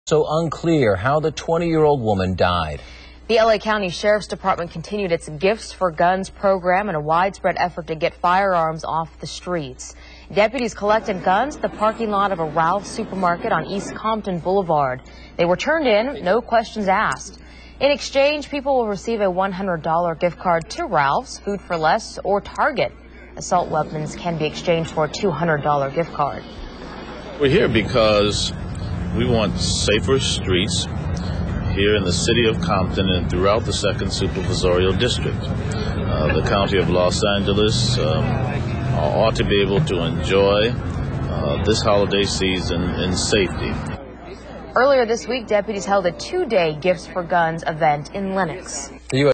Supervisor Ridley-Thomas Speaks About City of Compton Gifts for Guns Holiday Exchange Program
Supervisor Mark Ridley-Thomas talks to ABC 7 about keeping the Second Supervisorial District safe and the Compton Gifts for Guns holiday exchange program on 12/28/08.